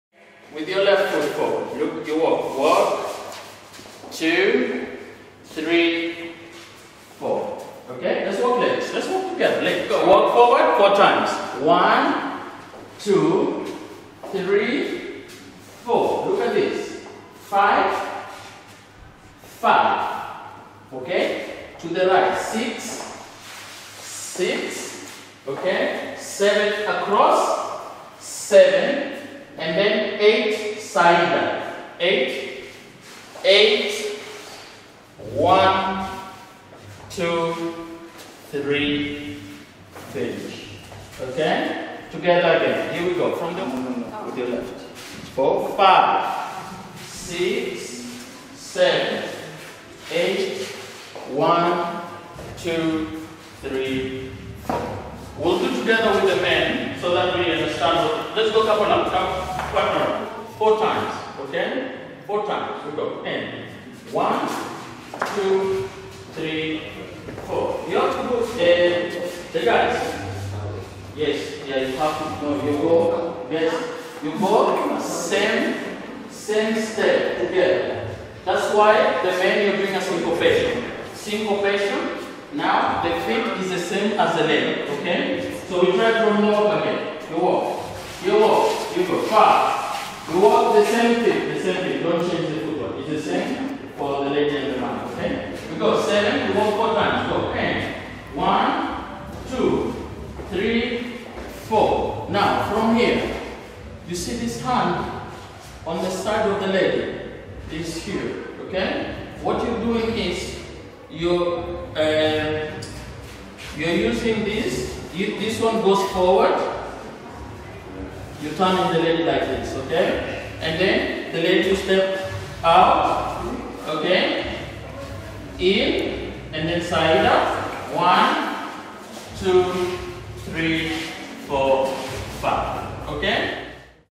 Kizomba Dance Workshop Basic Steps Mutati Dance Sardinia Festival 5th Edition Salsa Brisa
Kizomba Dance Workshop  Basic Steps  Mutati Dance
       Sardinia Festival 5th Edition Salsa Brisa